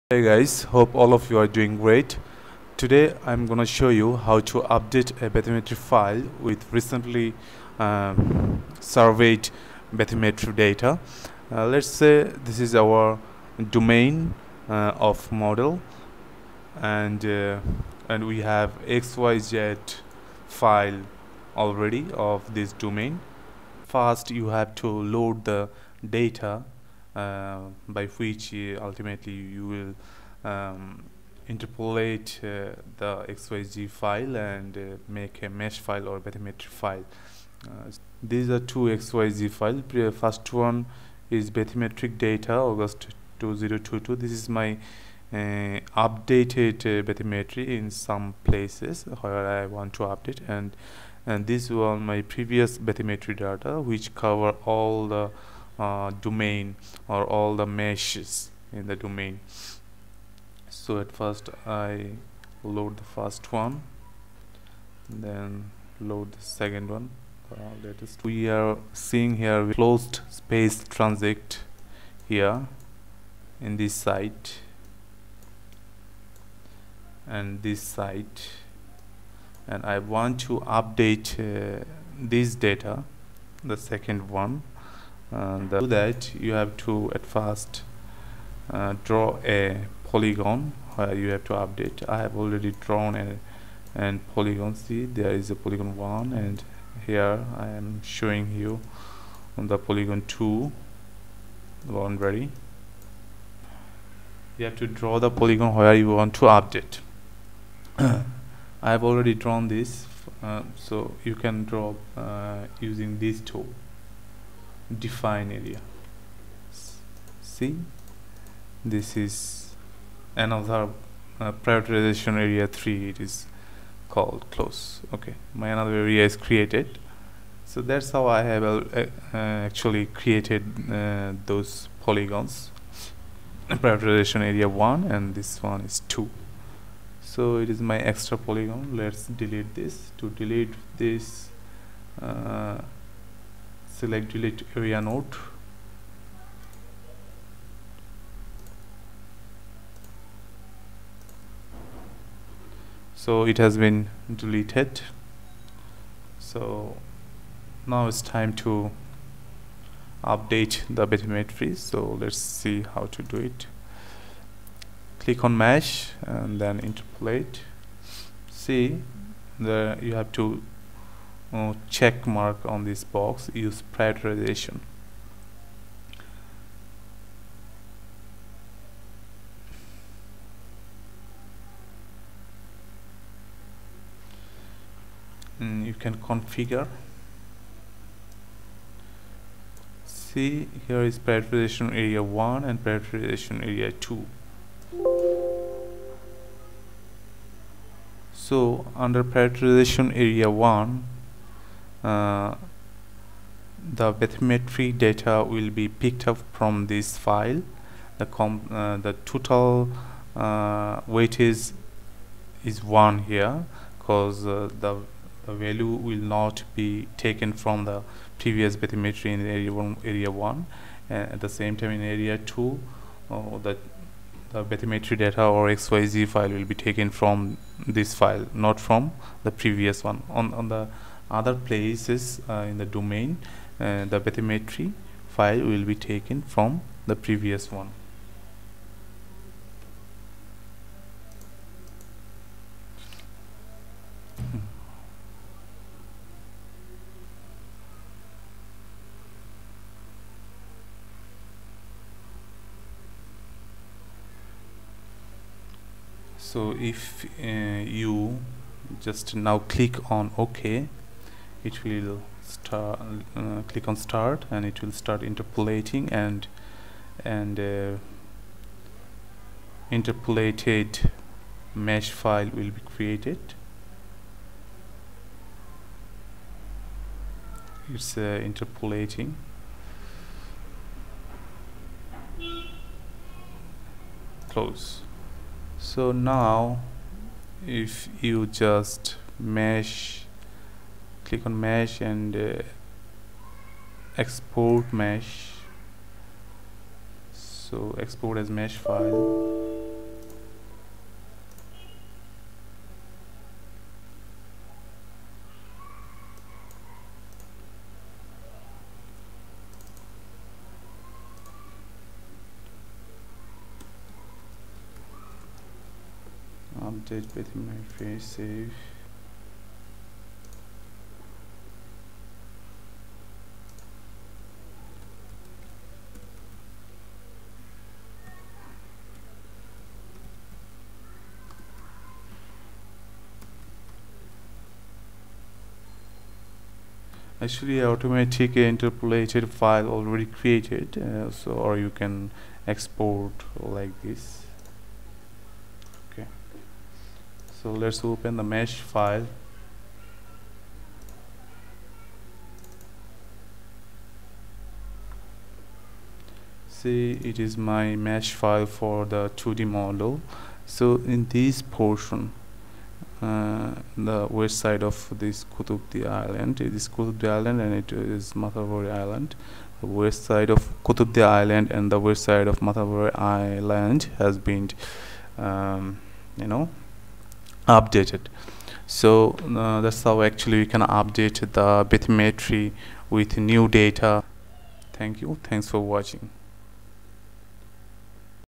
How to Update bathymetry in 2d mesh file by mdf file |MIKE FM | 2D HD Model | Dipen Saha
This is a tutorial video for MIKE 21 FM. Here I have shown how to update bathymetry with recent data or updated data step by step.